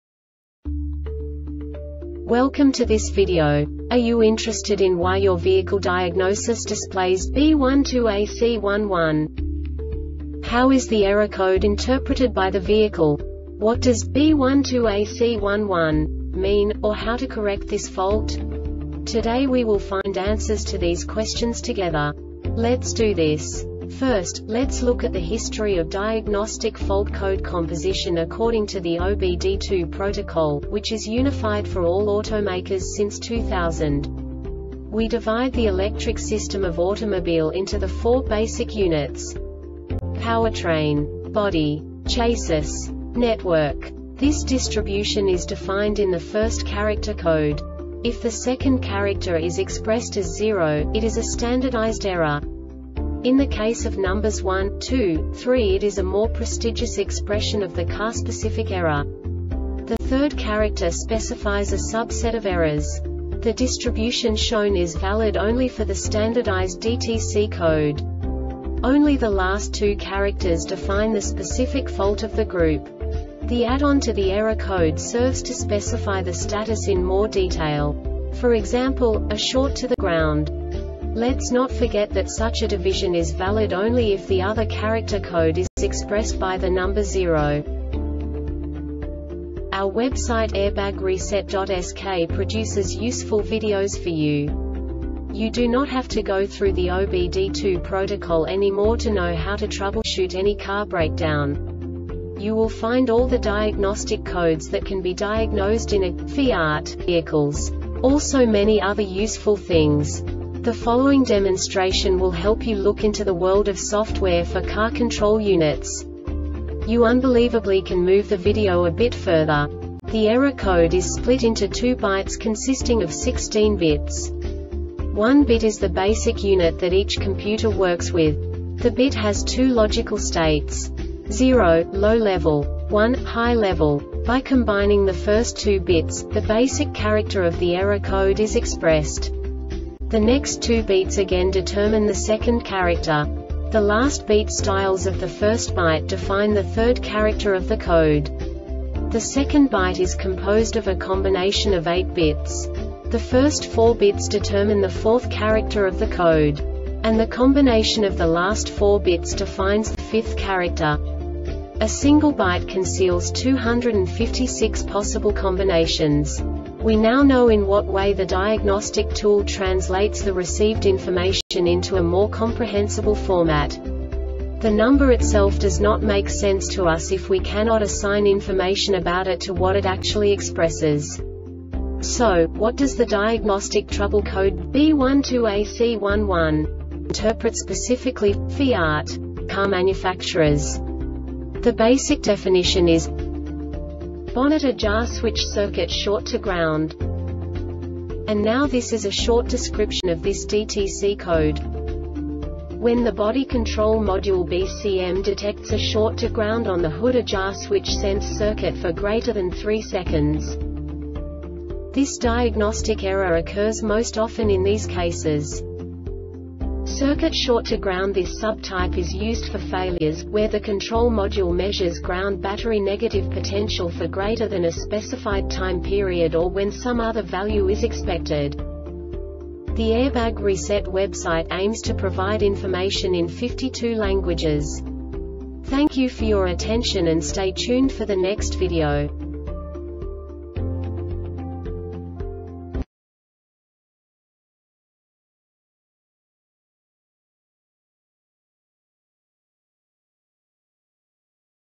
DTC Fiat B12AC-11 Short Explanation
Contents:
0:21 Basic DTC analysis according to OBD2 protocol standard.
1:48 Insight into programming
2:58 A diagnostically understandable description of the Diagnostic Trouble Code
3:05 Basic error definition

Make:
FIAT
Code:
B12AC-11
Definition:
Bonnet Ajar Switch-Circuit Short To Ground
Description:
When the Body Control Module (BCM) detects a short to ground on the Hood Ajar Switch Sense circuit for greater than three seconds.